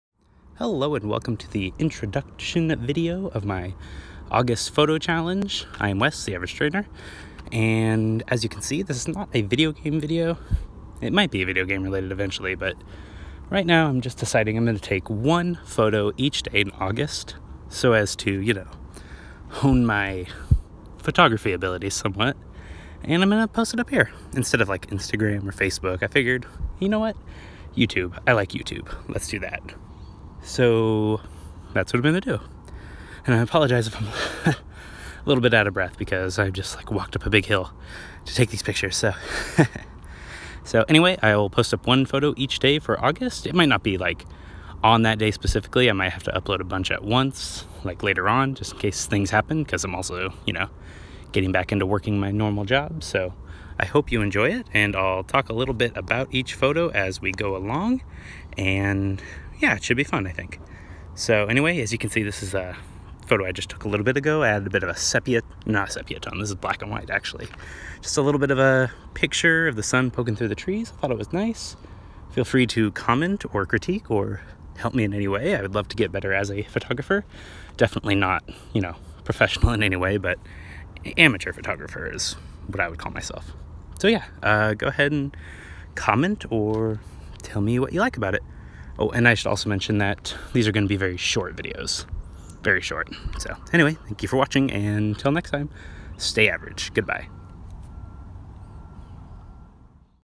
August 2017 Photo Challenge - Introduction
In this video, I talk a little bit about my August 2017 Photo Challenge!